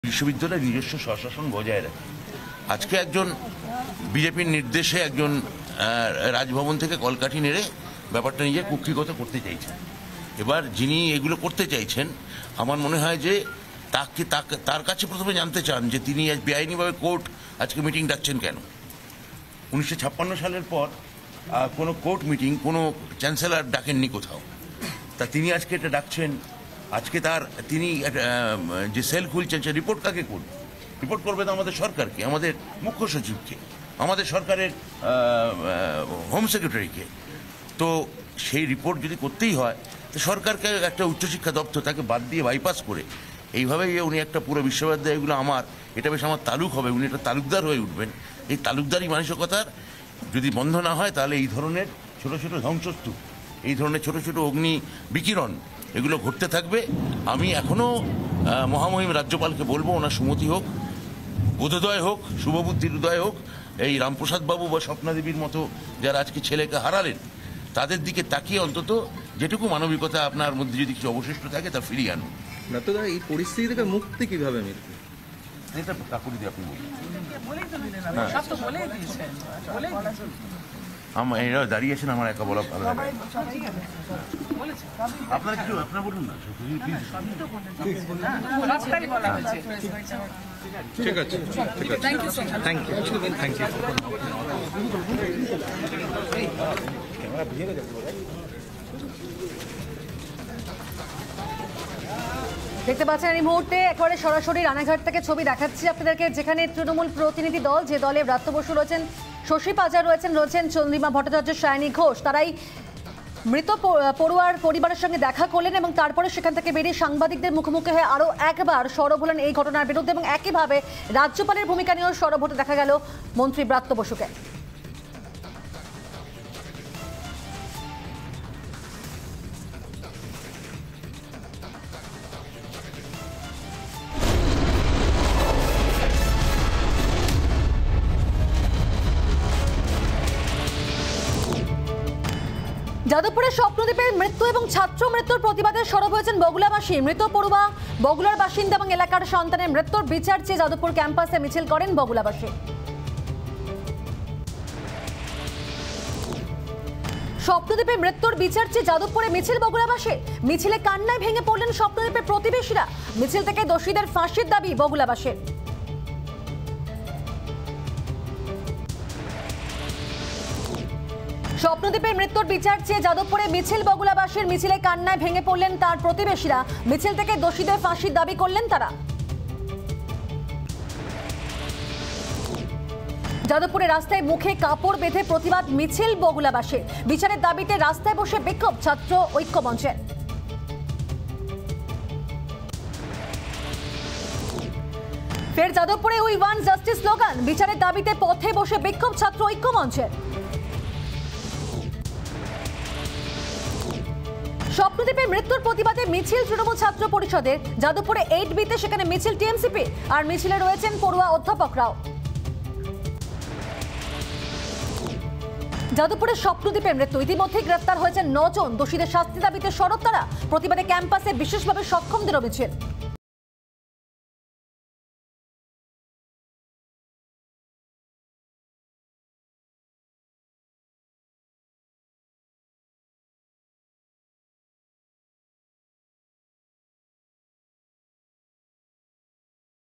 রানাঘাটে স্বপ্নদীপের মাামাবাড়িতে দাঁড়িয়ে রাজ্যপালকে তোপ শিক্ষামন্ত্রী ব্রাত্য বসুর
Jadavpur Incident News | ‘বিজেপির নির্দেশে রাজভবন থেকে কলকাঠি নেড়ে যাদবপুর বিশ্ববিদ্যালয়কে কুক্ষিগত করতে চাইছে।’রানাঘাটে স্বপ্নদীপের মাামাবাড়িতে দাঁড়িয়ে রাজ্যপালকে তোপ শিক্ষামন্ত্রী ব্রাত্য বসুর

রিপাবলিক স্বাধীন।
রিপাবলিক নির্ভীক।
আপনিই রিপাবলিক।